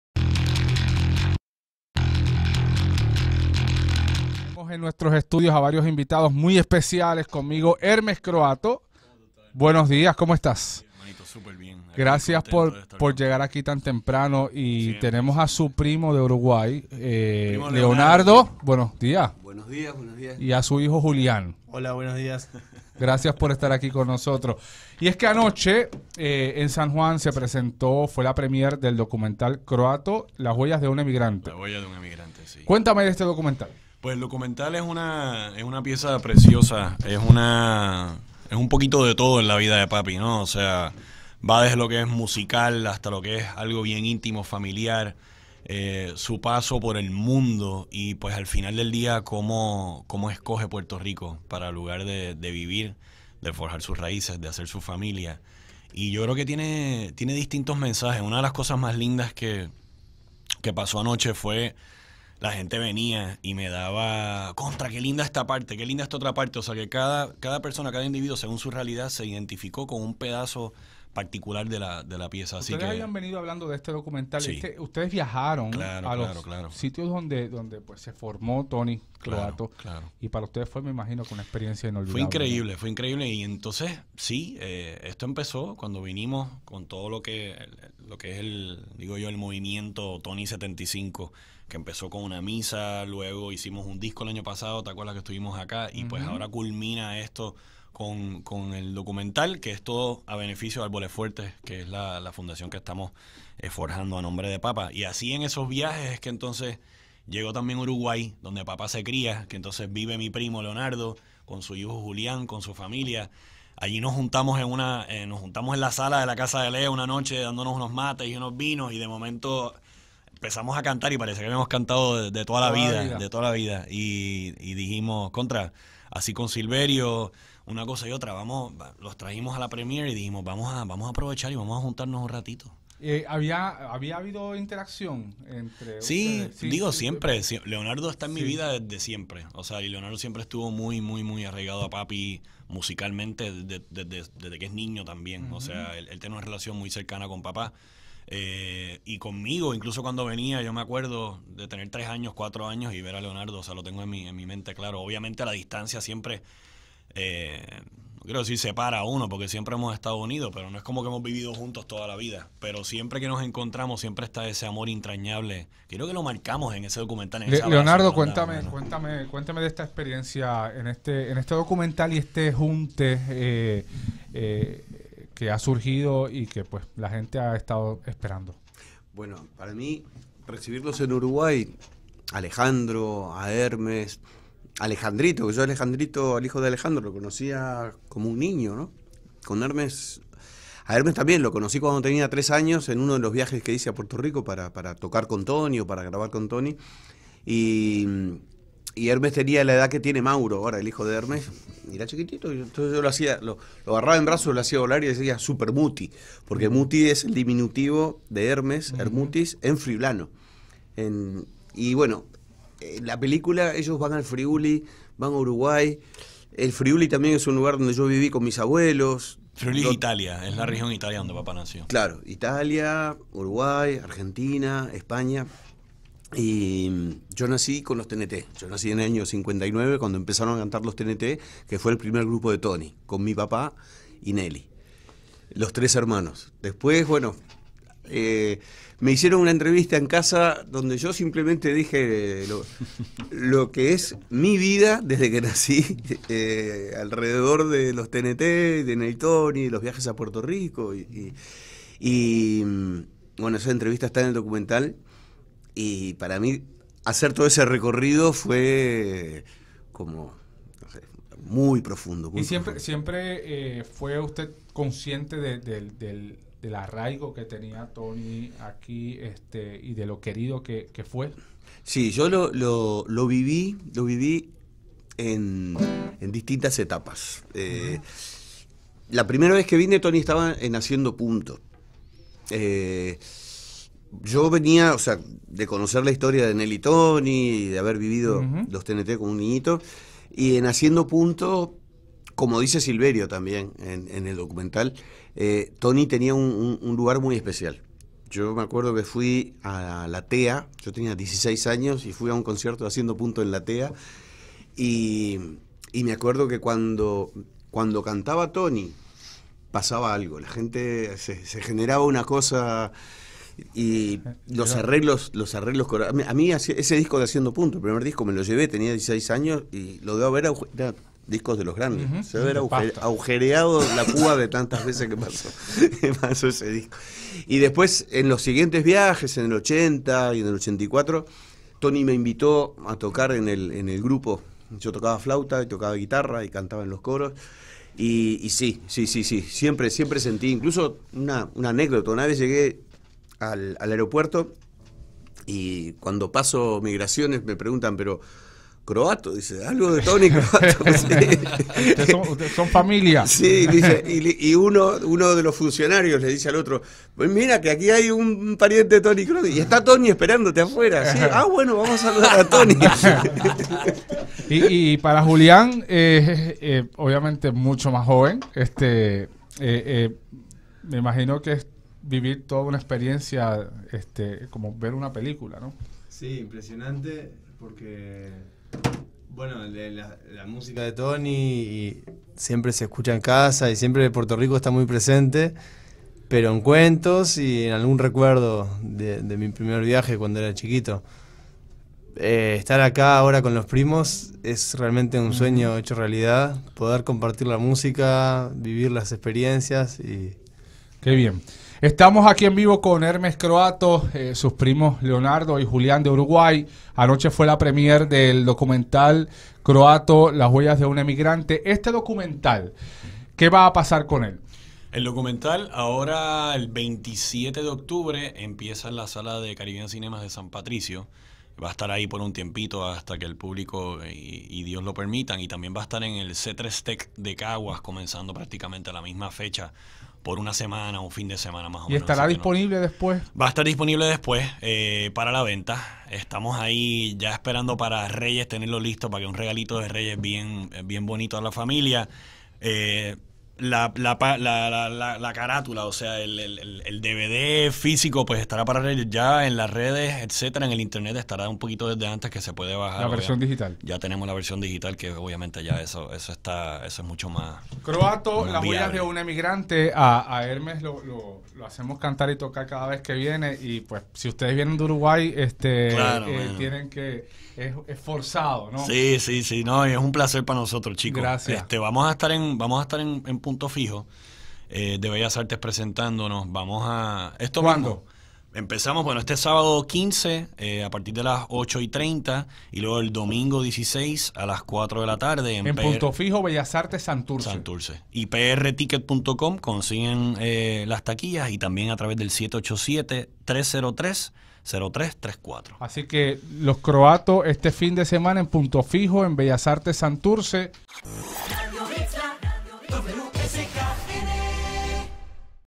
Croatto: la huella de un emigrante
Celebrarán la vida y el legado del cantautor Tony Croatto en un documental:Croatto: La huella de un emigrante.